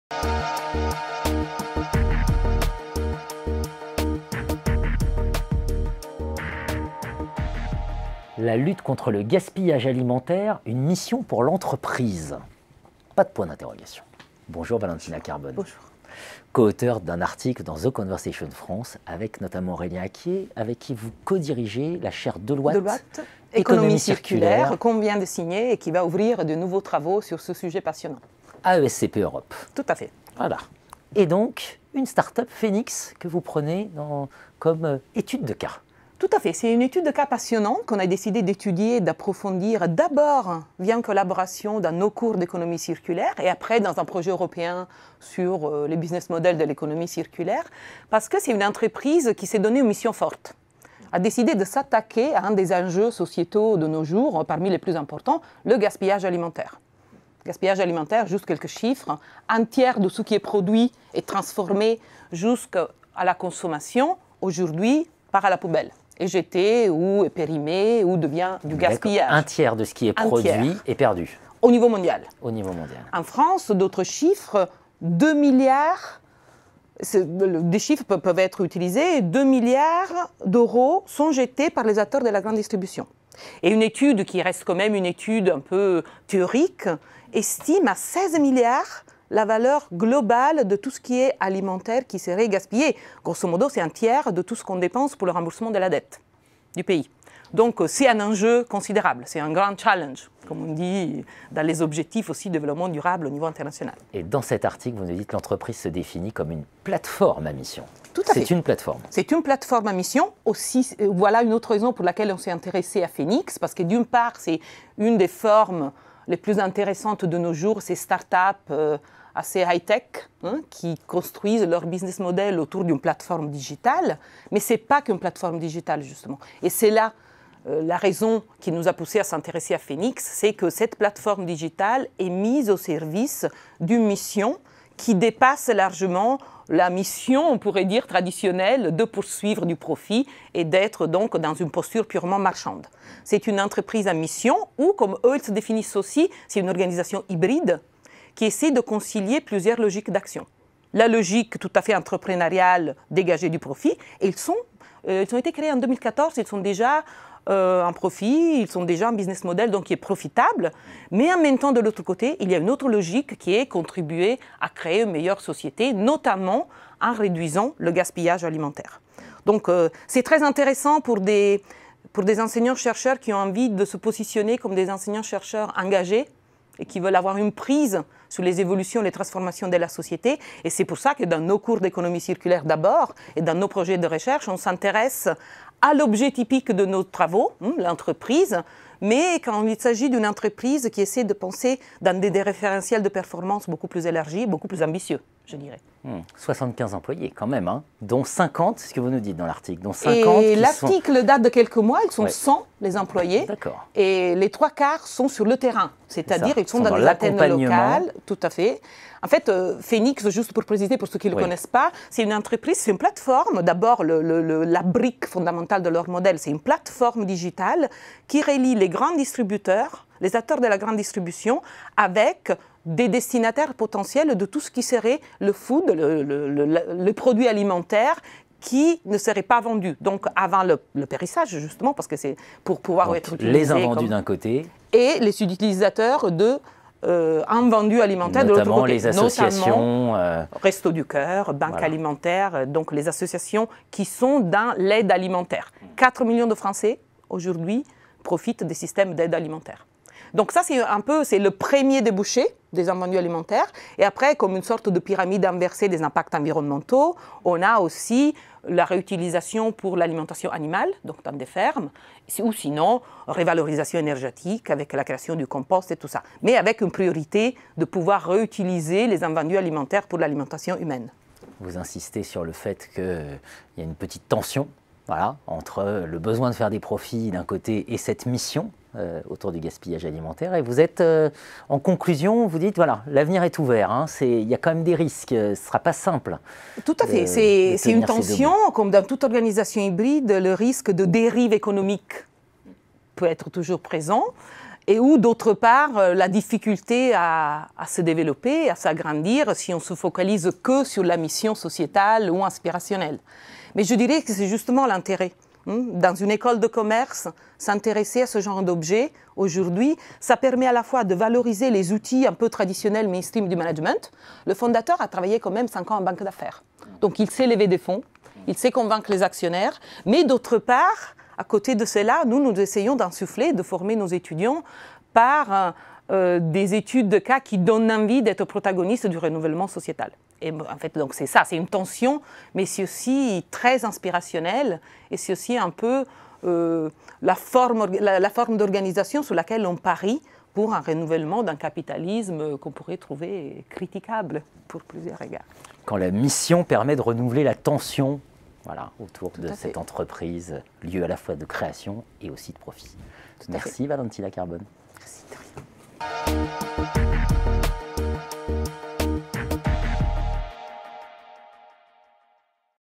Lutter contre le gaspillage alimentaire grâce à une plateforme numérique [Valentina Carbone]
Xerfi Canal a reçu Valentina Carbone, professeur à ESCP Europe, pour parler du gaspillage alimentaire.